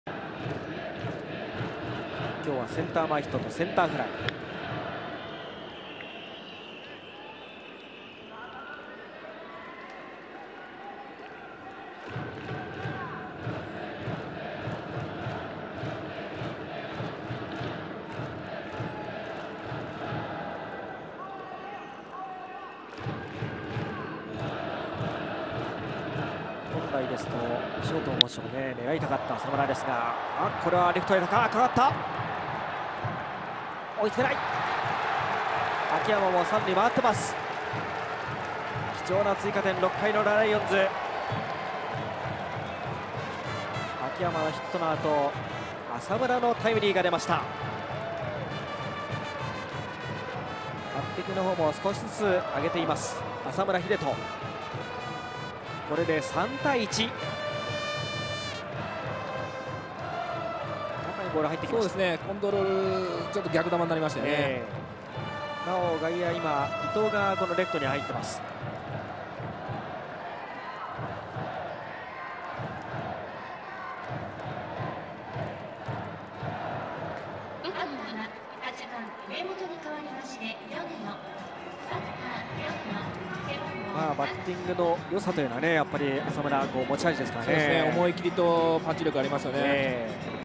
浅村が大きな一打！貴重なタイムリーで阪神を引き離す 2013.03.17 L-T
6回、埼玉西武は浅村のタイムリーで1点を追加し、阪神との差を2点に広げた。 （3月17日 埼玉西武-阪神）